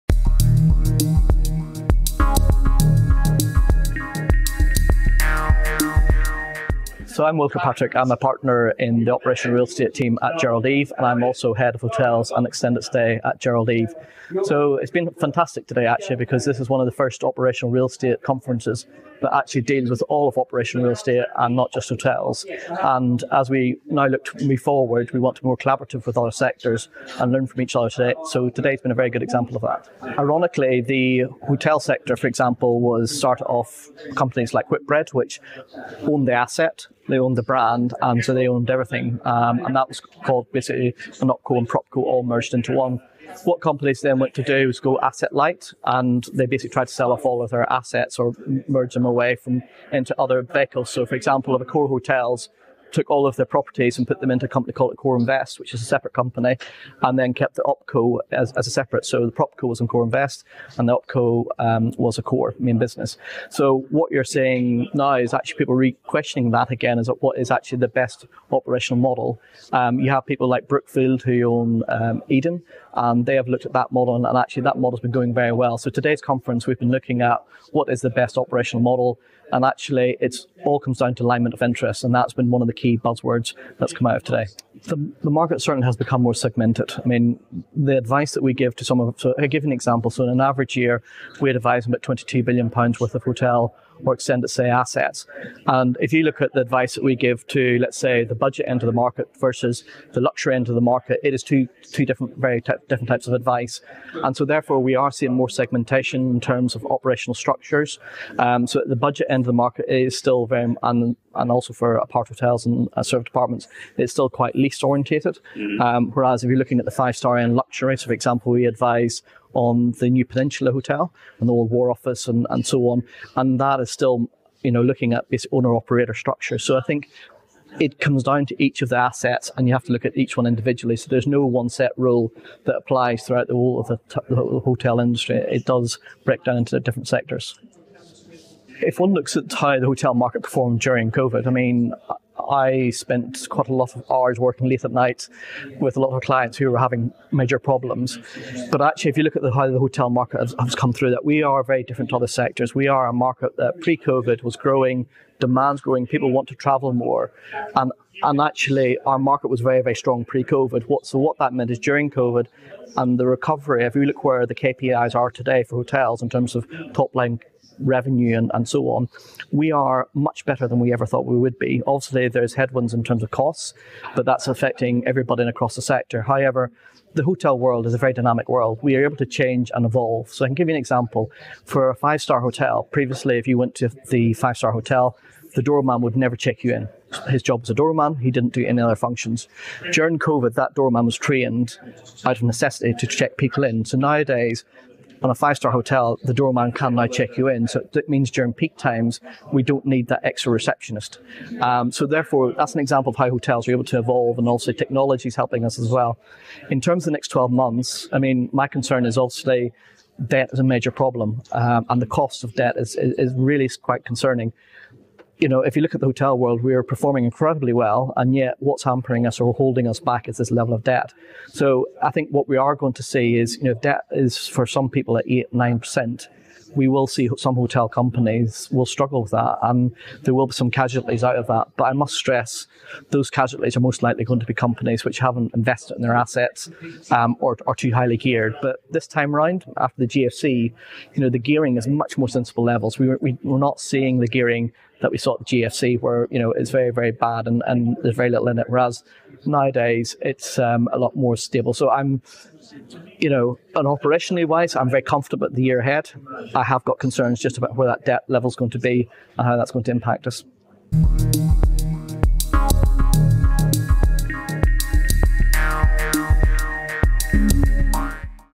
William Kirkpatrick OpRE22
OpCo, PropCo and BrandCo...where next?  Will Kirkpatrick of Gerald Eve explains. Will was interviewed at OpRE 2022 in London.